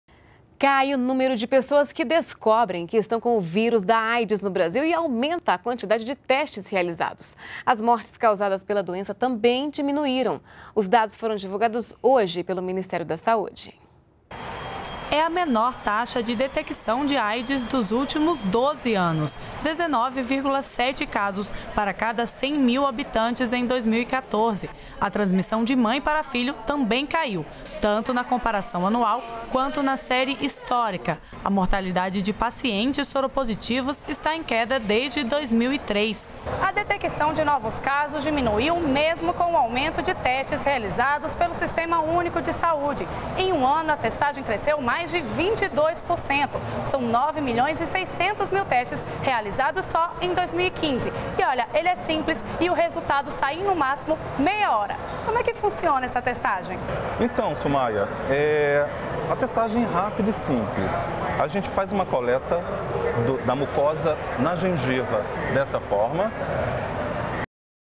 Ministério da Saúde divulga Boletim Epidemiológico de HIV e Aids 2015
NBR NOTÍCIAS - 1º.12.15: Ministério da Saúde divulgou nesta terça-feira (1º) o Boletim Epidemiológico de HIV e Aids de 2015, o balanço aponta dados de taxa de detecção e mortalidade pela doença no país. Segundo o boletim, a taxa de detecção de aids caiu 9%, de 21,6 casos em 2003, para 19,7 por 100 mil habitantes em 2014. A redução é a maior nos últimos 12 anos de epidemia.